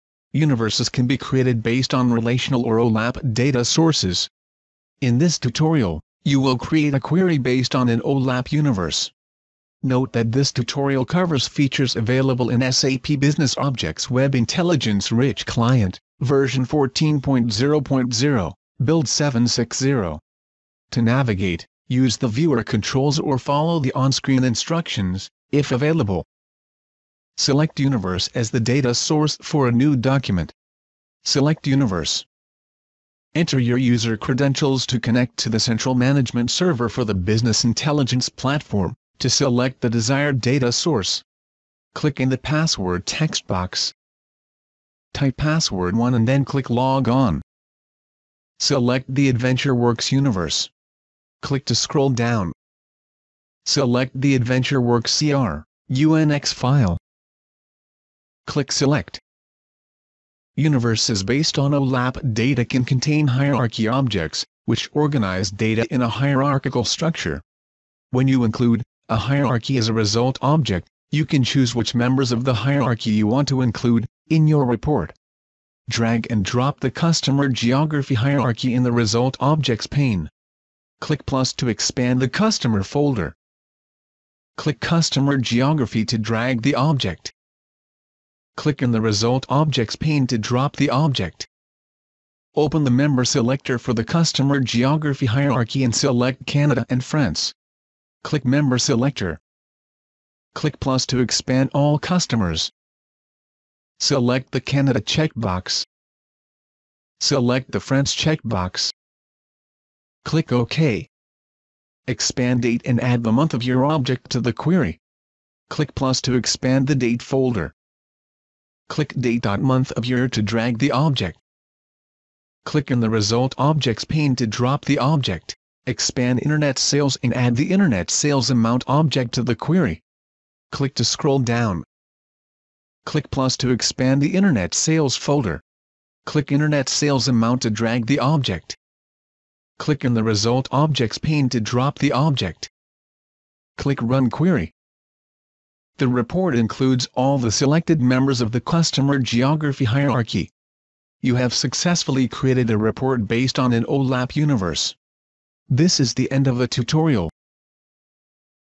Create a query based on an OLAP universe: Web Intelligence 4.x
Web Intelligence 4.0: For the interactive version of this tutorial, along with other SAP BusinessObjects BI Suite tutorials, visit us